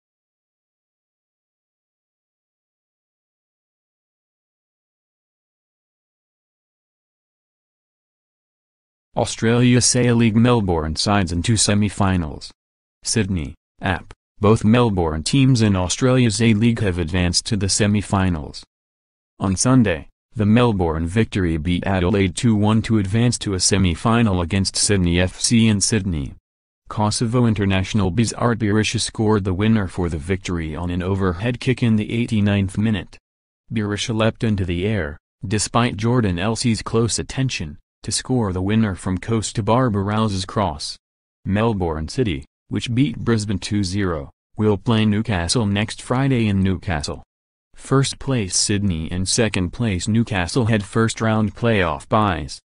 Australias A League Melbourne sides into semifinals
SYDNEY (AP) — Both Melbourne teams in Australia's A-League have advanced to the semifinals.

On Sunday, the Melbourne Victory beat Adelaide 2-1 to advance to a semifinal against Sydney FC in Sydney. Kosovo international Besart Berisha scored the winner for the Victory on an overhead kick in the 89th minute.

Berisha leapt into the air, despite Jordan Elsey's close attention, to score the winner from Kosta Barbarouses' cross.